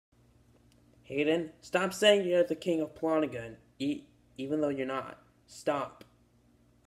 Angry message to Hayden G! 😠😠😠 (YES, THAT WAR IS STILL GOING ON!)
This guy is stupid, he literally just started a war by saying he’s the king of Plotagon even though he’s not!

Friend me on Nintendo Switch: SW-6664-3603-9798